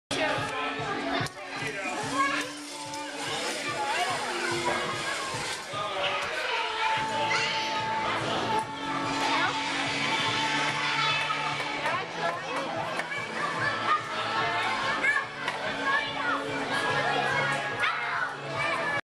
Taylor's BDay Party 5
Taylor's third trip down the zip-line at Jungle Quest